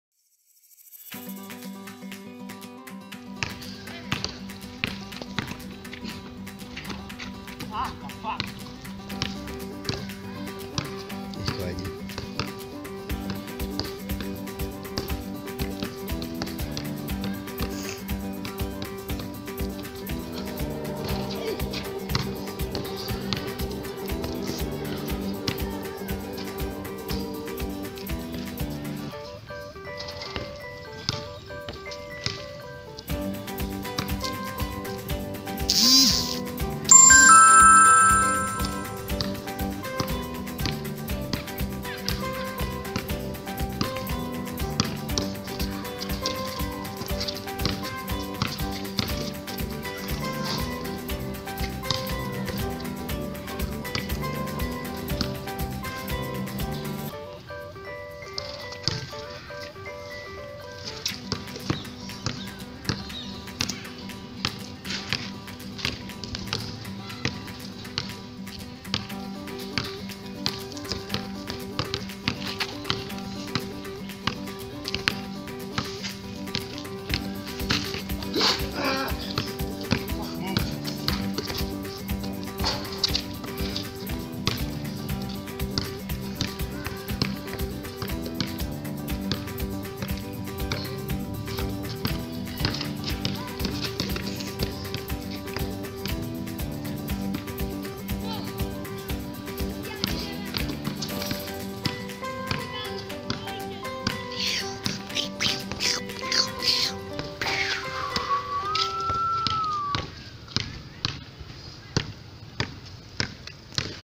Day one one on one
This trying to go one on one when tired Kenyan style you have to try not to loose .